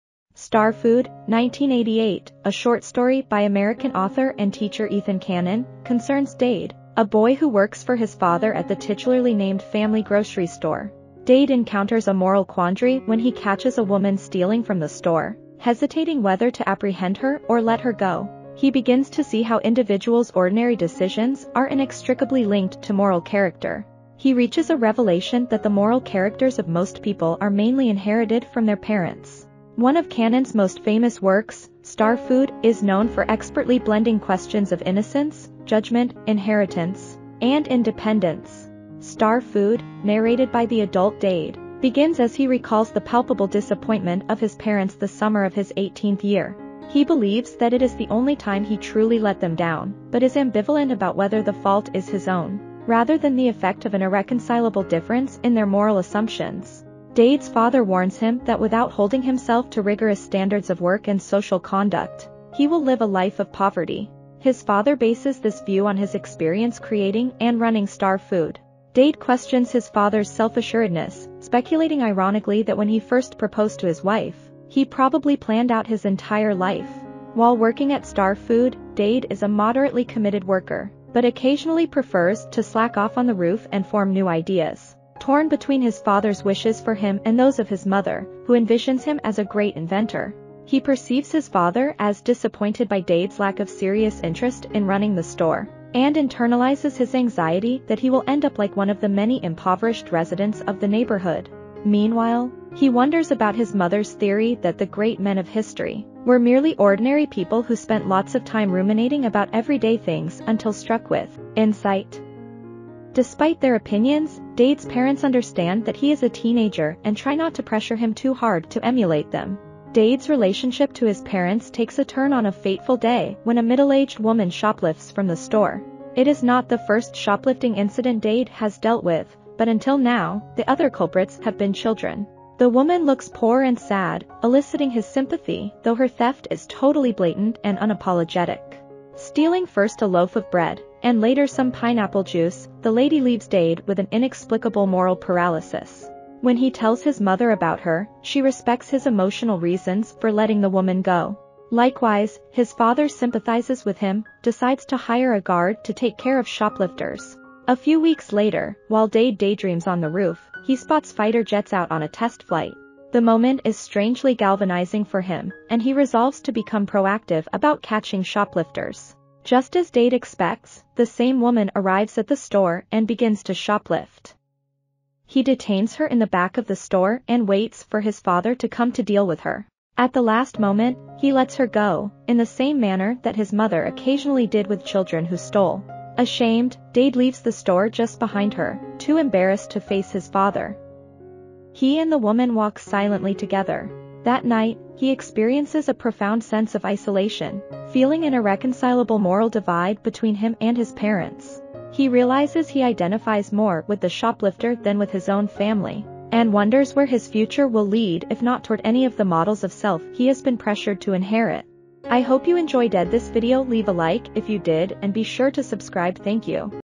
Plot summary, “Star Food” by Ethan Canin in 3 Minutes - Book Review
"Star Food" is a short story by American author Ethan Canin. It follows the life of a young boy named Eddie, who is growing up in a working-class family in a small Midwestern town. Eddie's father, who works at a local factory, is obsessed with the idea of providing his family with the best possible life, including the finest foods. He becomes fixated on a brand of canned peaches called "Star Food," which he believes will make them a better family. The story explores Eddie's complex relationship with his father, as well as his own struggles to understand the world around him. As Eddie grows older, he begins to see the limitations of his father's worldview, and realizes that there is more to life than material possessions. The story is a poignant exploration of family dynamics, social class, and the complexities of growing up. Canin's writing style is known for its emotional depth and keen observations of human behavior. "Star Food" has been widely anthologized and is considered one of Canin's most celebrated works.